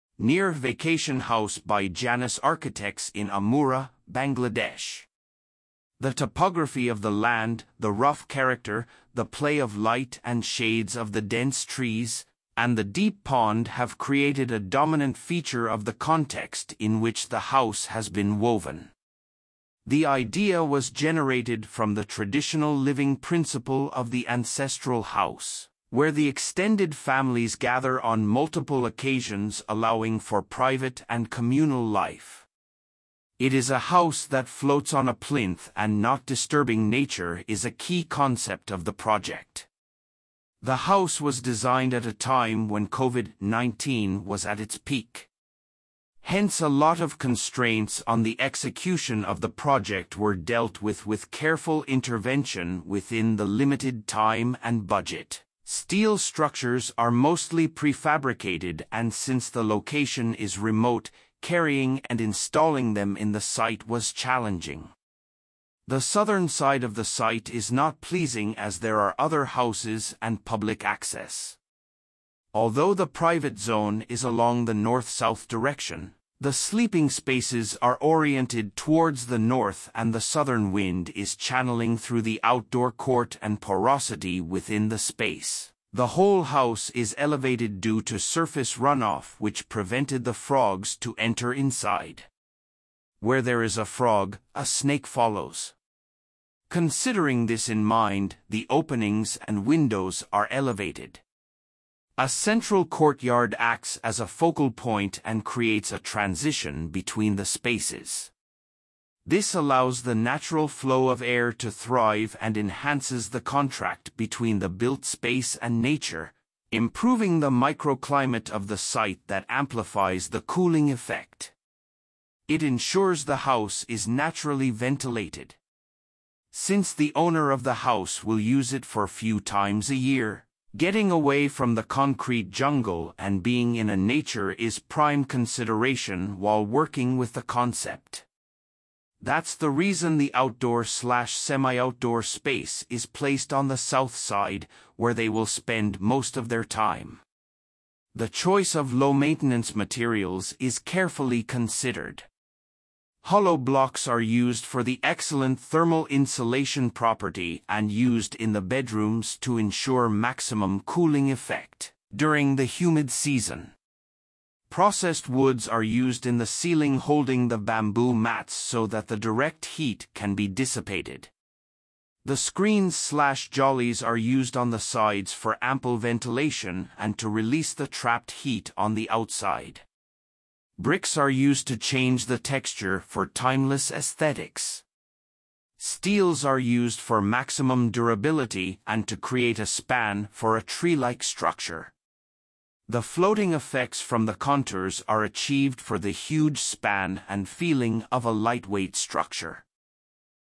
‘Neer’ Vacation House By Janus Architects In AMURA, BANGLADESH
The topography of the land, the rough character, the play of light and shades of the dense trees, and the deep pond have created a dominant feature of the context in which the house has been woven. The idea was generated from the traditional living principle of the ancestral house where the extended families gather on multiple occasions allowing for private and communal life. It is a house that floats on a plinth and not disturbing nature is a key concept of the project.

Archi Studio

Architects: Janus Architects
Area: 2674 ft²
Year: 2021
Photographs:Asif Salman
Manufacturers:  DBBL, Lamination, Lighting World, PMG, RAK, Timber
Structural Consultant: One stop Design Solution
Interior Design: Janus Architects
Lead Architect Design Concept, Management: Zishan Fuad Choudhury
Associate Architect Design Drawing Development: Shadman Saadat
Documenatation: Arfan Ahmed Purno, Esrat Jahan Priya, Sudipta Mondal
Program / Use / Building Function: Vacation House
City: Amura
Country: Bangladesh